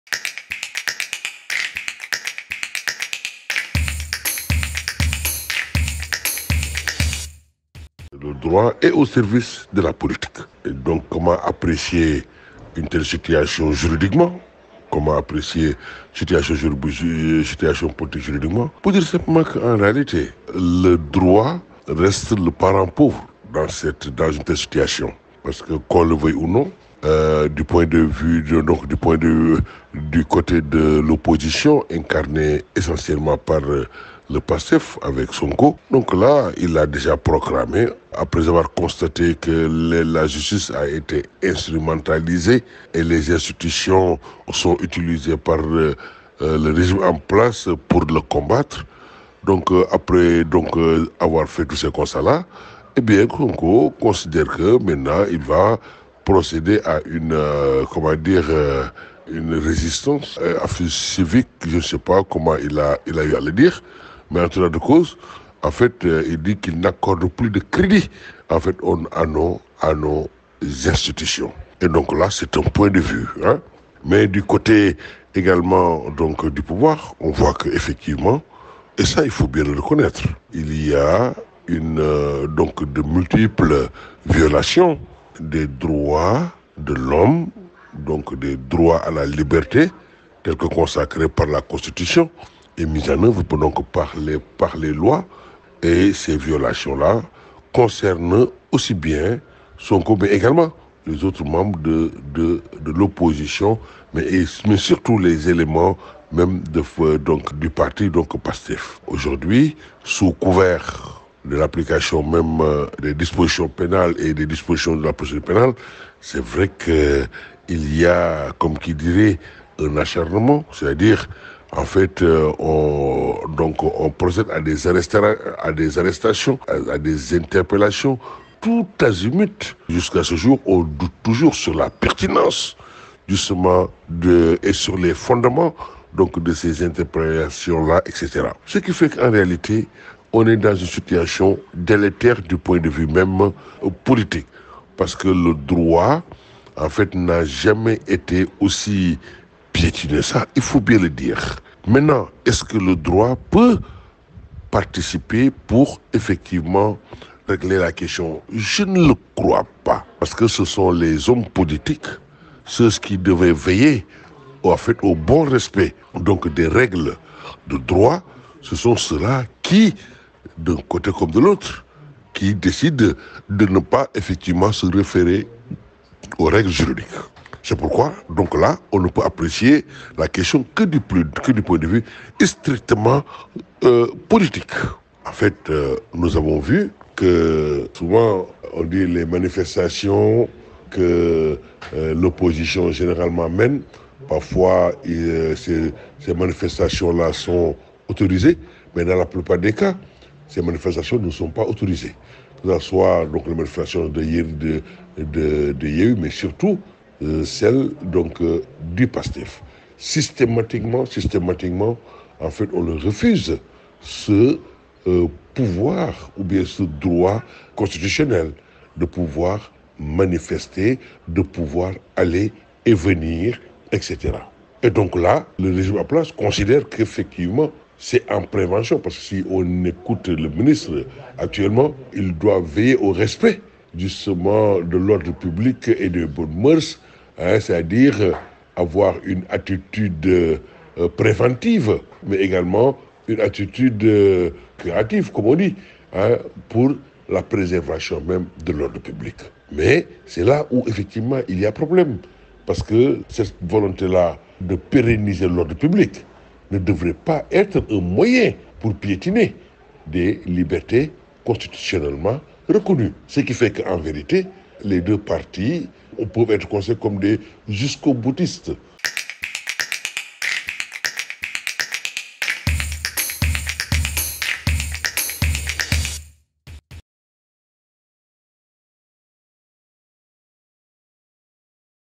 SÉNÉGAL-TENSION POLITIQUE : LE DROIT N'EST PAS LA SOLUTION !
La tension politico-judiciaire qui secoue le Sénégal ne connaîtra pas de solution par le Droit. C'est la lecture de Iba Barry Kamara. Aux yeux de l'enseignant-chercheur, les parties prenantes n'ont de cesse de piétiner le Droit. Extrait 2 de son décryptage sur wanneltv